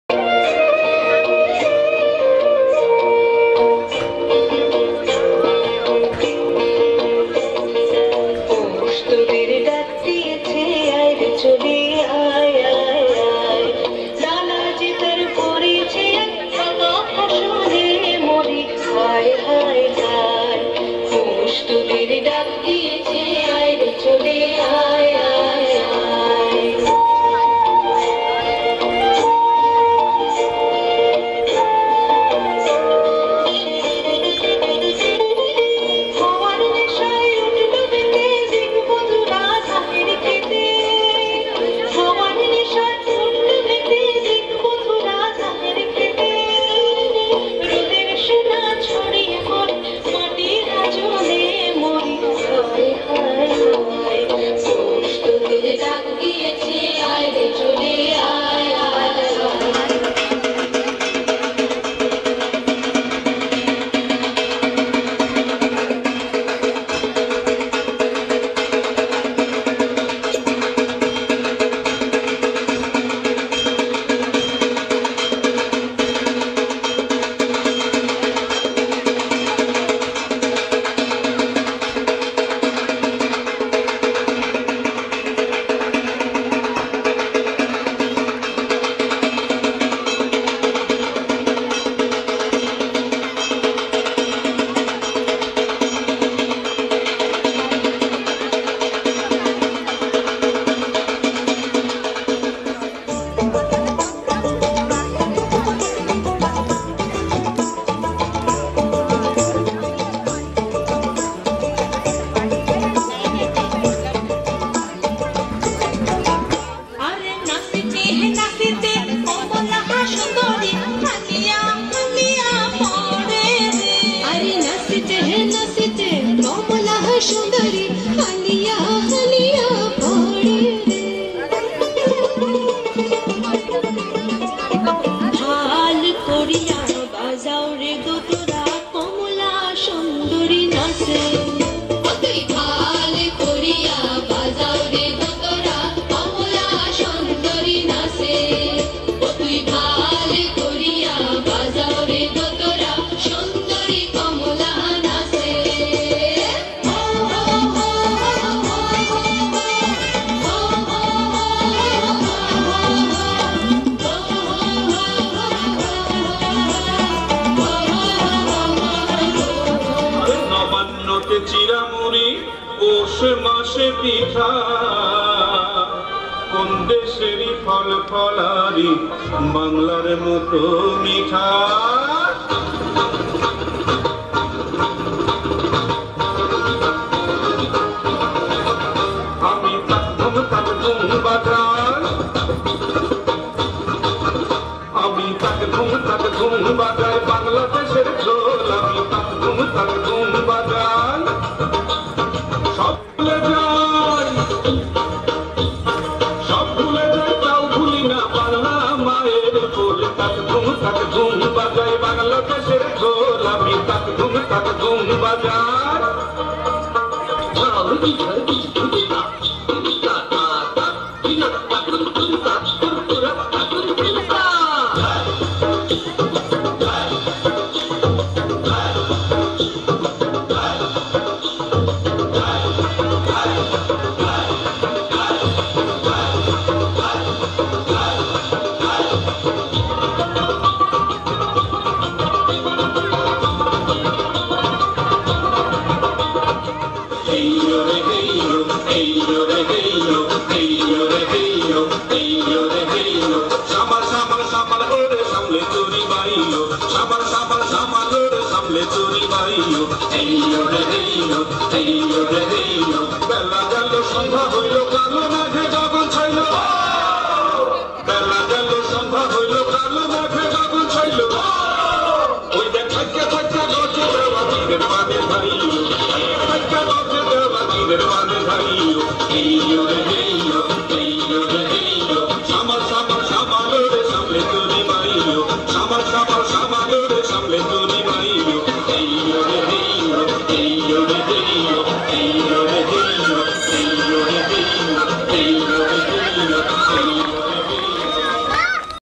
IEFF 2019 India Reena Das Dance Group (Day 1)
IEFF-2019 : India Reena Das Dance Group- DAY-1 (Oct 18th)
International Festival of Ethnic Folklore - 2019
Place: New Delhi , INDIA
Dates: Oct 16-21, 2019